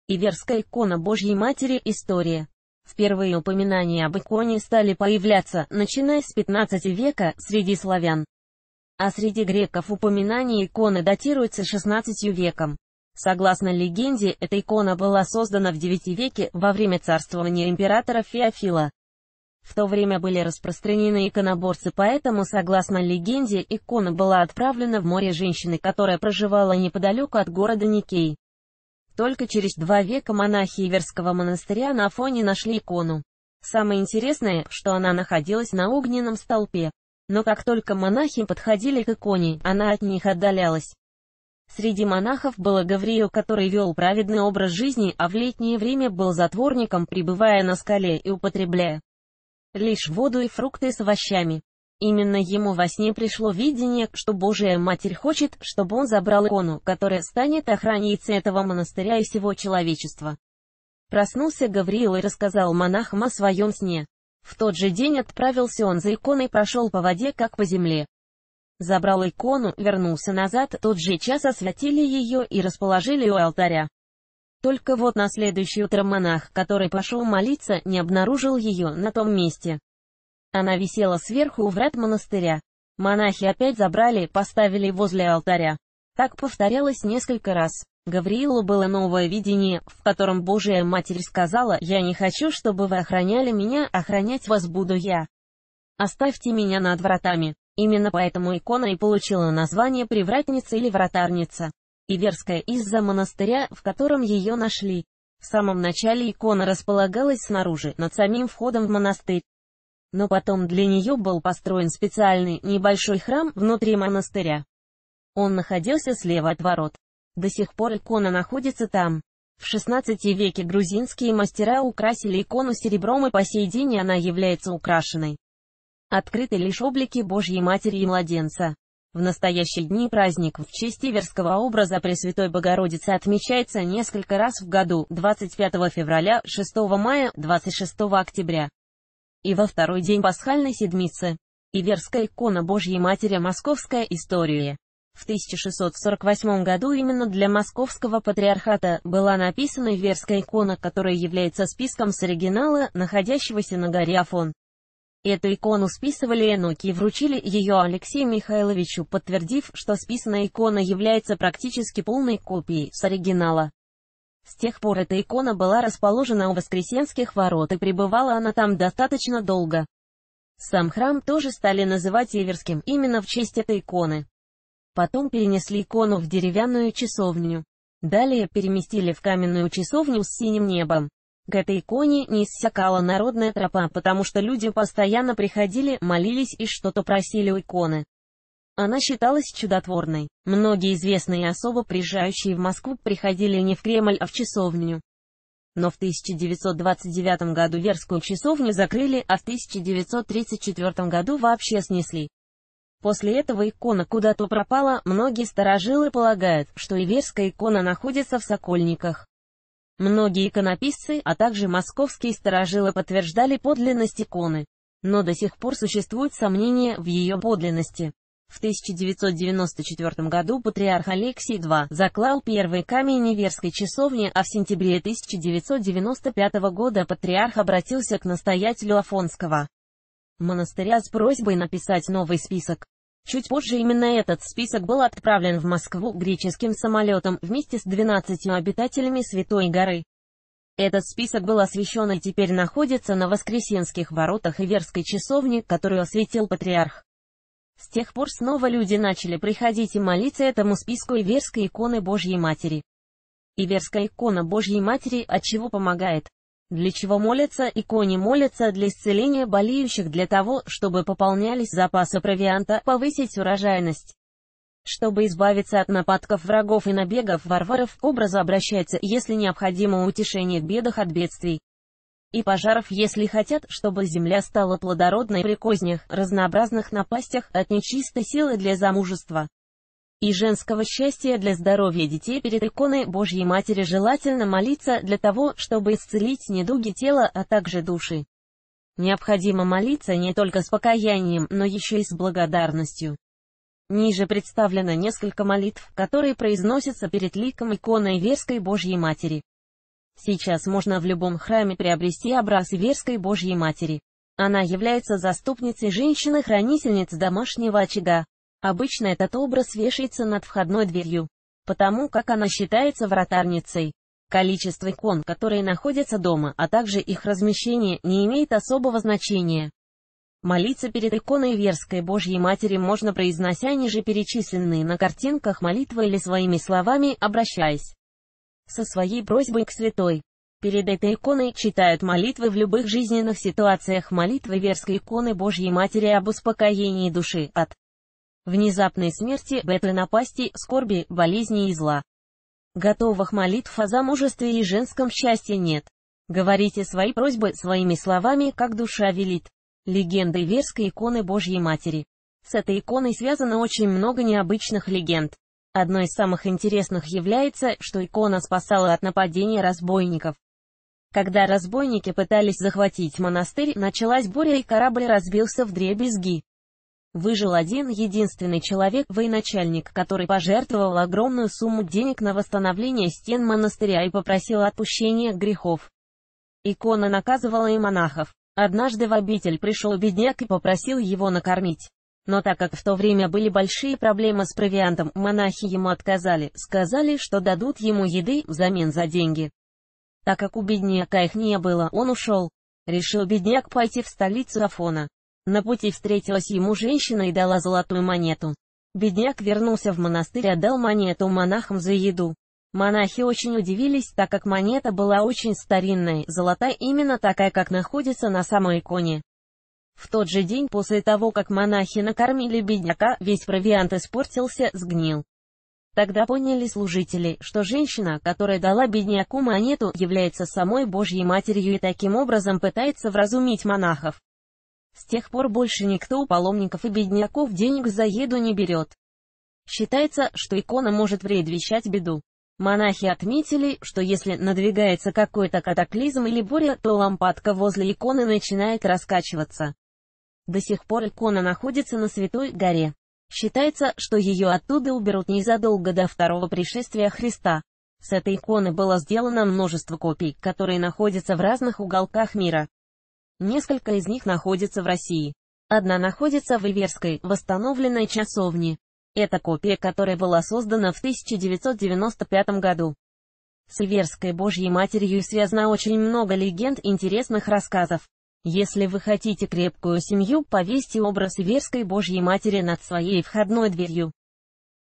Легенды, история и фото Иверской иконы Божией Матери. От чего помогает Иверская икона Божией Матер.
Легенды, история и фото Иверской иконы Божией Матери. От чего помогает Иверская икона Божией Матери, и как ей надо молиться?